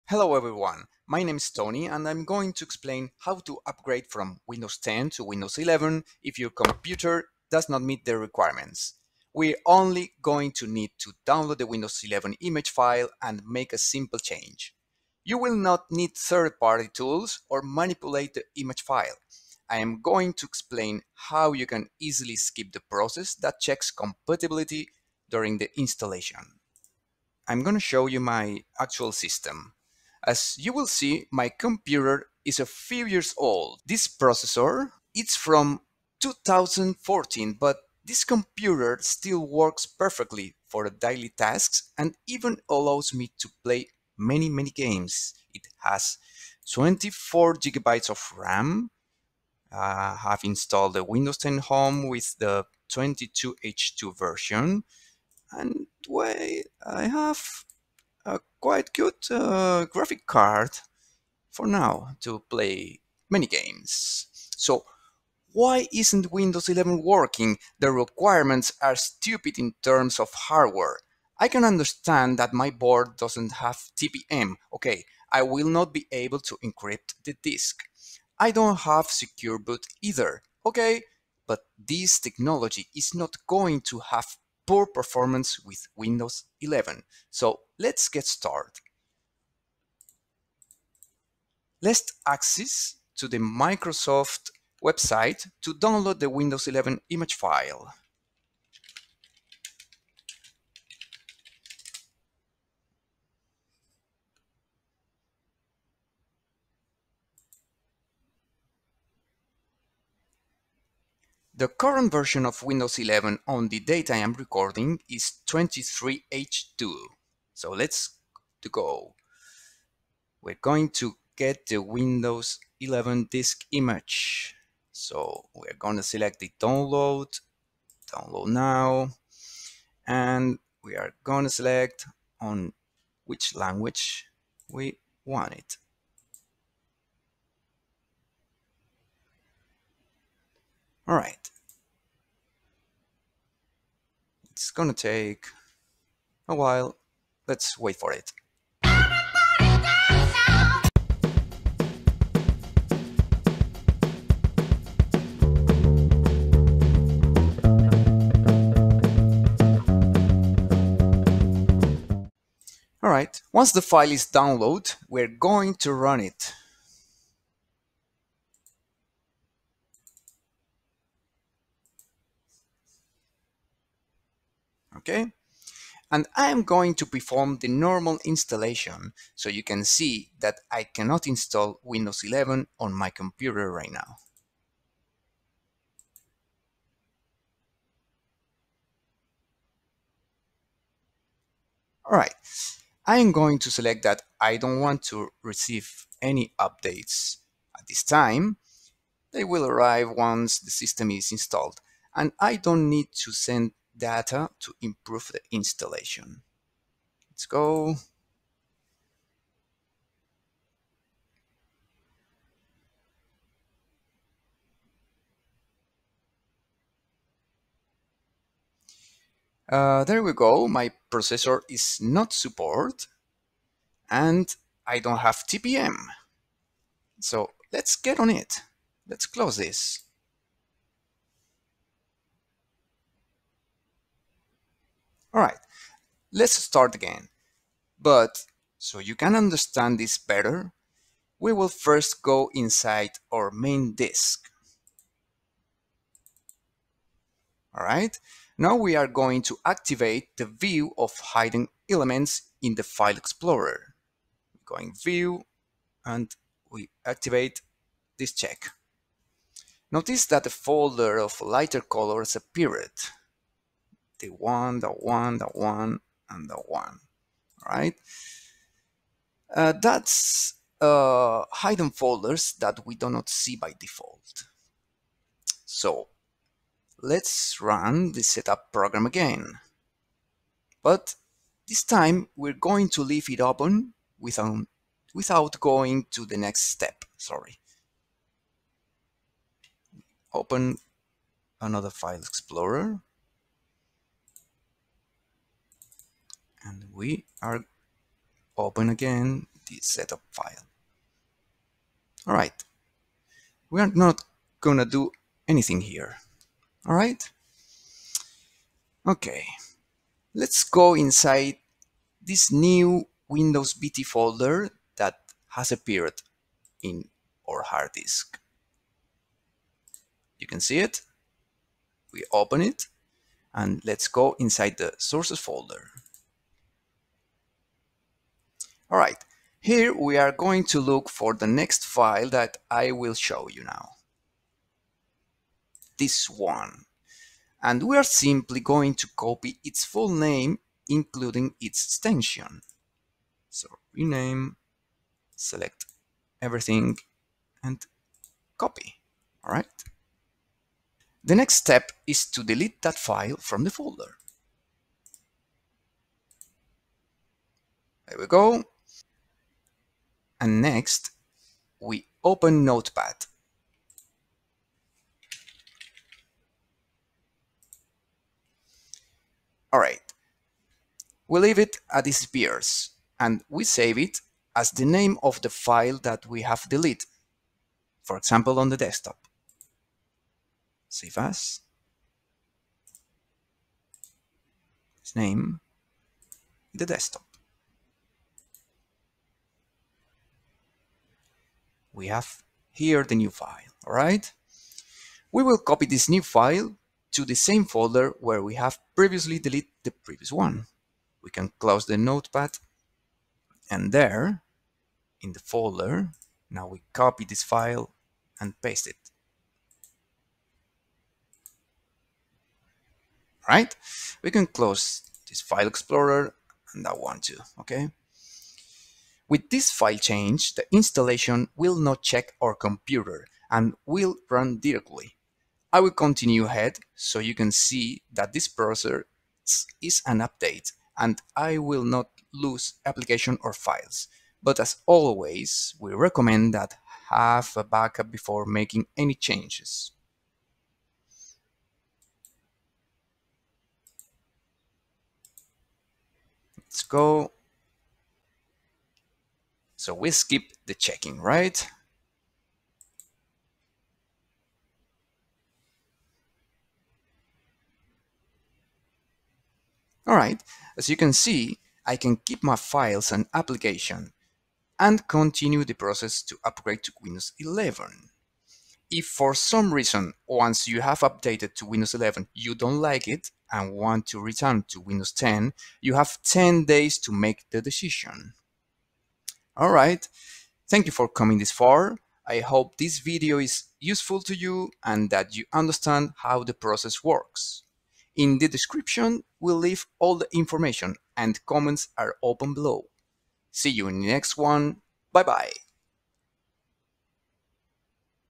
Upgrade to Windows 11 on Unsupported Hardware (EASY METHOD) v.23H2
Want to upgrade to Windows 11 but your PC doesn't meet the hardware requirements?

 In this video, I'll show you an easy method to bypass the system requirements and install Windows 11 on unsupported devices. This step-by-step guide will walk you through the entire process, from preparing your computer to installing the operating system. Whether you have an old laptop, a desktop PC, or a low-end machine, you can follow these simple instructions to upgrade to Windows 11 without any hassle. So, don't let outdated hardware hold you back from experiencing the latest features and security updates of Windows 11.